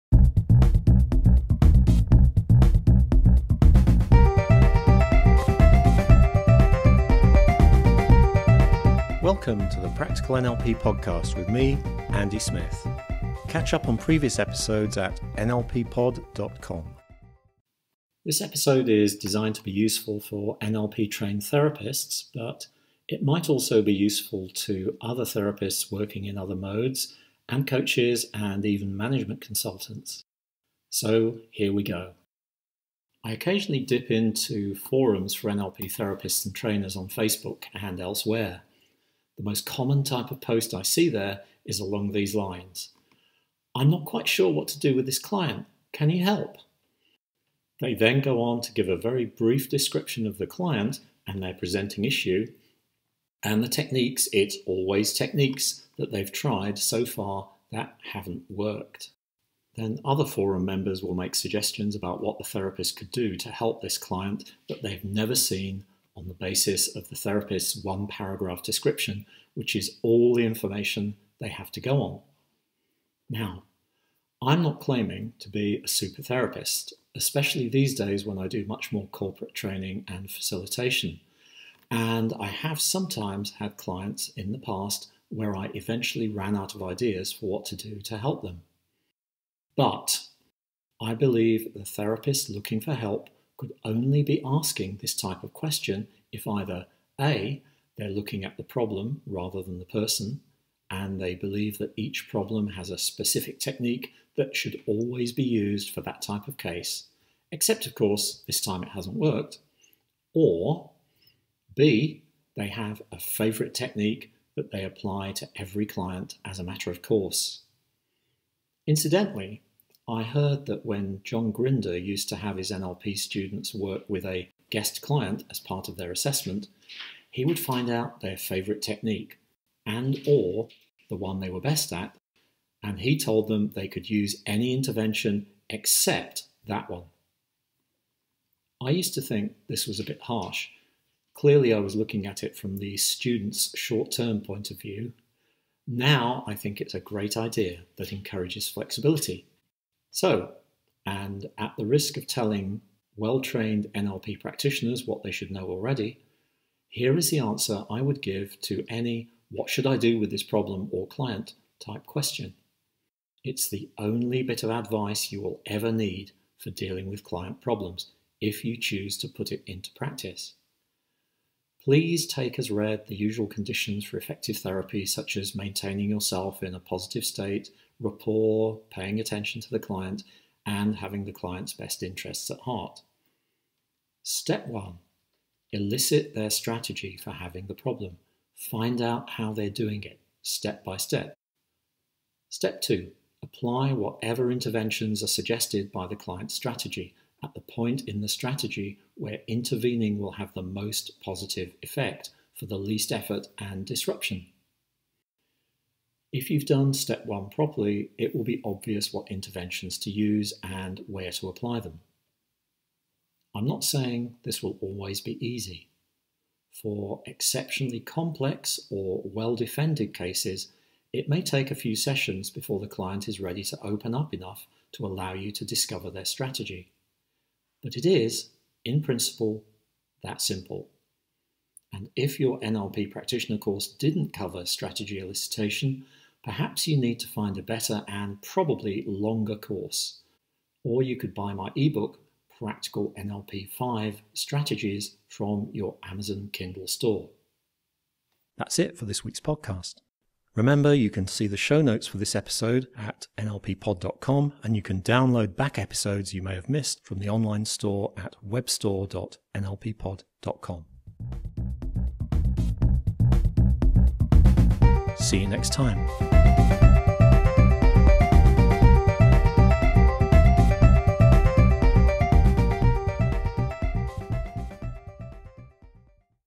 Practical NLP Podcast 87 - The Only Advice You'll Ever Need For Helping Clients Solve Their Problems
A bit of a rant this week, inspired by the many, many posts I've seen on NLP forums from rookie NLP practitioners who ask what technique they should be using with a particular client problem.

They're going about it all wrong, and their NLP trainers should have showed them the right way!

Here's how to approach any client problem so you'll know what to do.